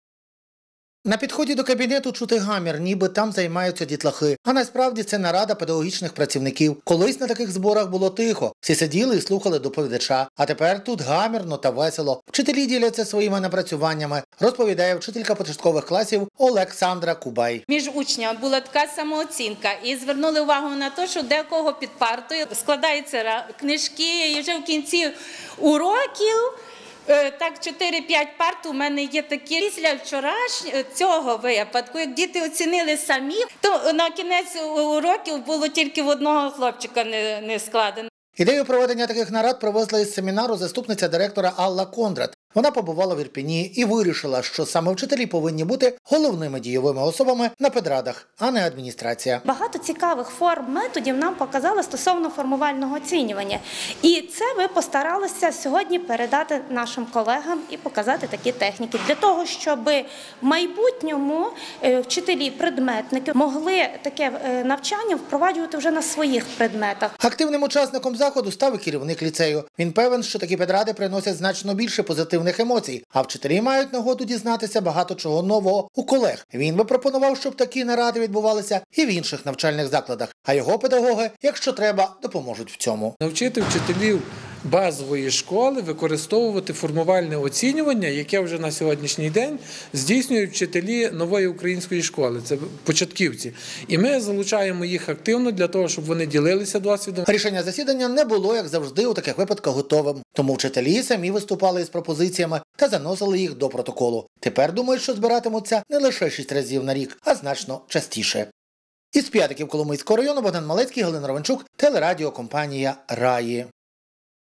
На Коломийщині викладачі ліцею перетворили нудні наради на гру
На педраду не як на каторгу тепер ходять вчителі П’ядицького ліцею. Адже цей захід відбувається тепер у формі гри. Викладачі працюють в групах та діляться досвідом. І якщо колись всі спостерігали за годинником, чи бува вже не закінчення то тепер і розходитися не хочуть.
Більше про це розкажуть наші коломийські кореспонденти.